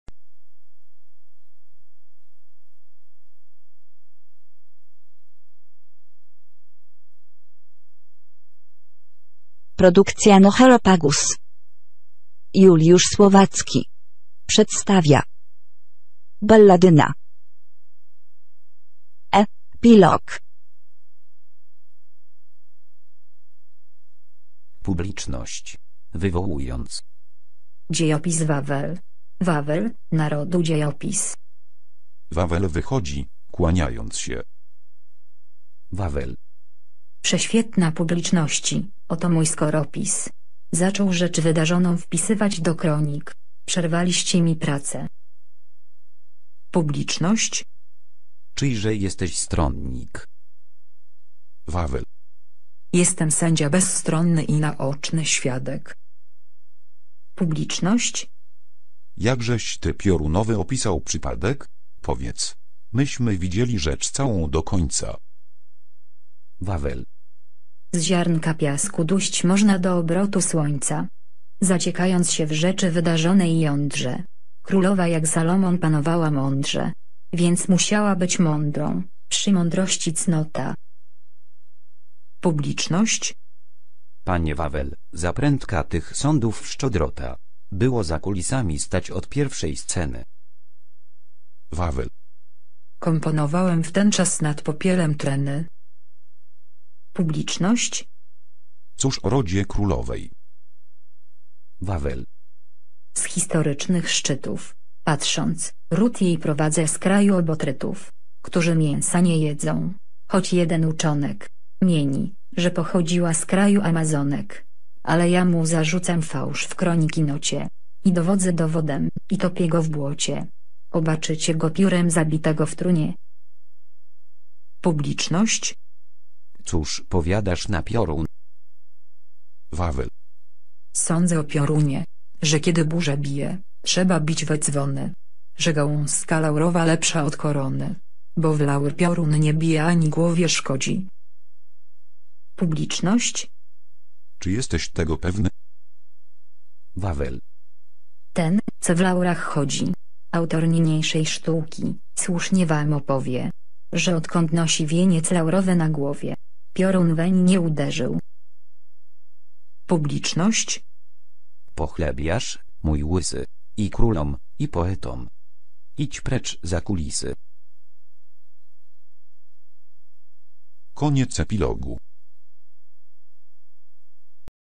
-' Balladyna '- Epilog -
Juliusz Słowacki przedstawia utwór pod tytułem "Balladyna" .
Czyta syntezator .